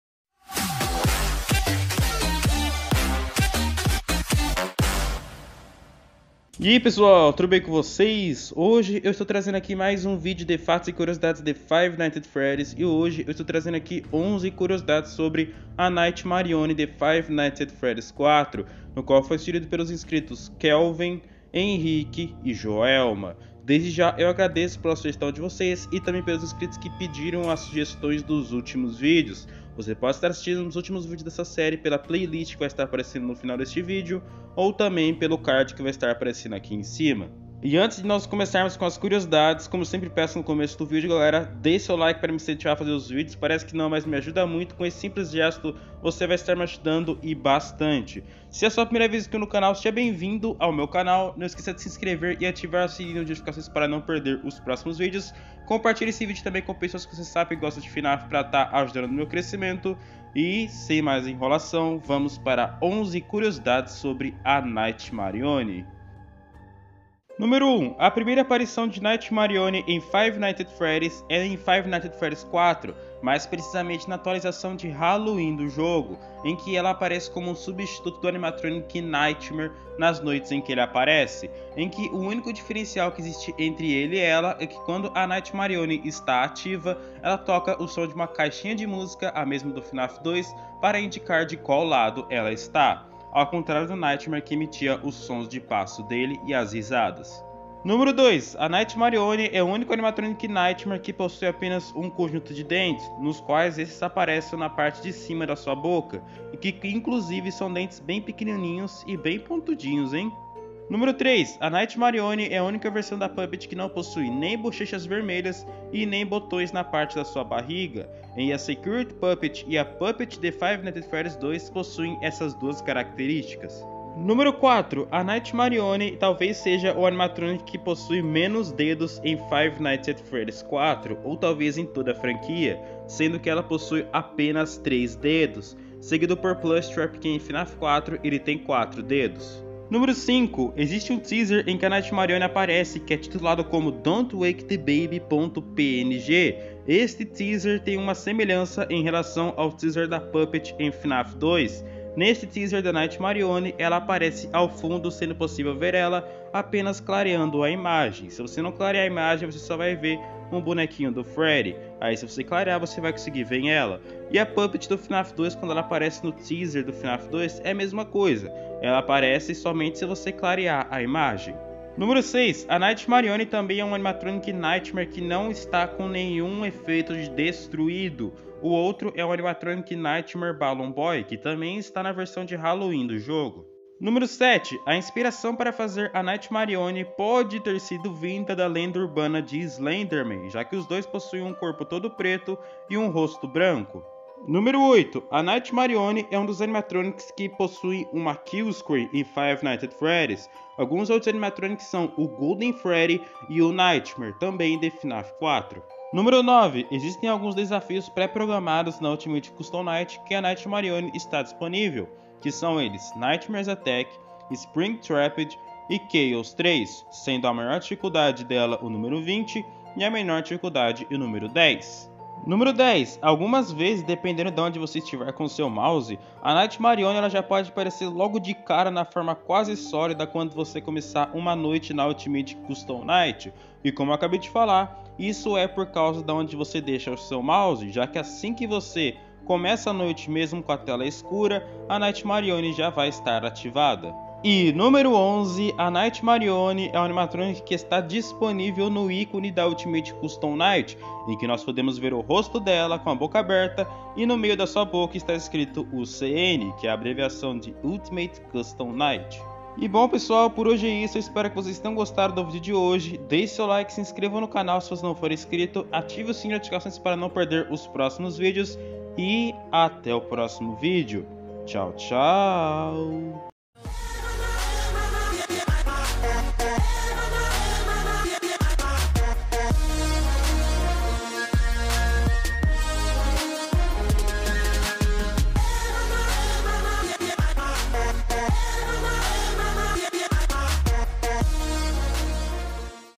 11 CURIOSIDADES da NIGHTMARIONE!
•Sobre este vídeo:

  A Nightmarione é um animatronic presente em Five Nights at Freddy's 4, na versão de Halloween. Ela substitui o Nightmare nas noites em que ele aparece.

  Neste vídeo, estou trazendo 11 Curiosidades sobre ela, fique até o final para não perder nada!

Basta dar o play e curtir o vídeo à vontade ▶️

📍 Timeline do vídeo📍

00:00 - Introdução

01:07 - Curiosidades

04:19 - Encerramento

📍Playlists recomendadas📍

📍Produção📍

• Capa do vídeo feita por Renan Alves.

~Discord dele: renansr#5856

• Edição:  Luiz Eduardo "Luigio"

• Roteiro: Luiz Eduardo "Luigio"

• Voz: Luiz Eduardo "Luigio"